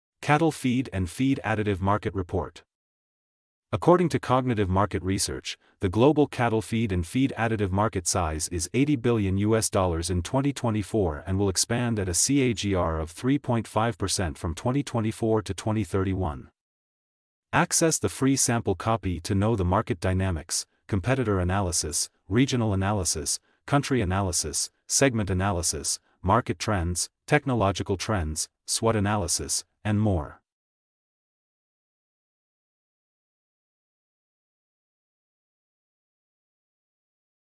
Cattle Feed and Feed Additive Market Report 2024
Cattle Feed and Feed Additive Market Report 2024 | Forecast, Market Size & Growth

Report Insights: According to Cognitive Market Research, the global Cattle Feed and Feed Additive Market size is USD 80 billion in 2024 and will expand at a compound annual growth rate (CAGR) of 3.5% from 2024 to 2031